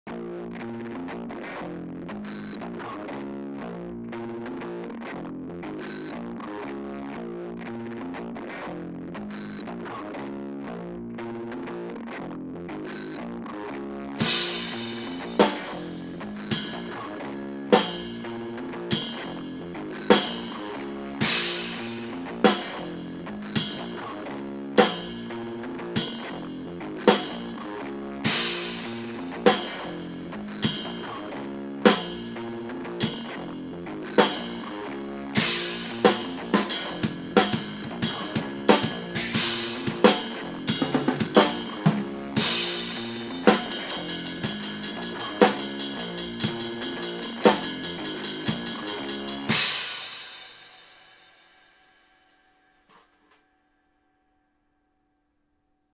Money Challenge (Metric Modulations)
A smorgasbord of metric modulations and nested tuplets.  Don't forget them quints! let me know if you want this loop it's super fun to play with!